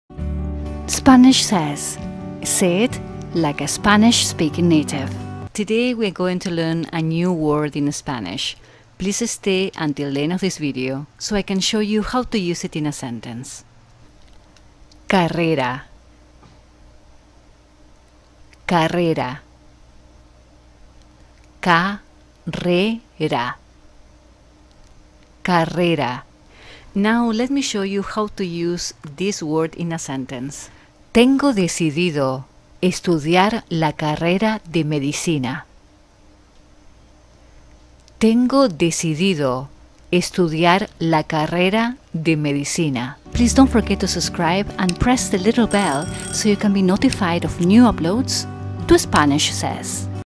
Carrera: How to Say / Pronounce Carrera - Career in Spanish | Spanish Says
How to say "Carrera" (Career) in Spanish. Make sure to watch the whole video for the example sentence as well. La pronunciación de "Carrera" en español con una oración de muestra para su uso.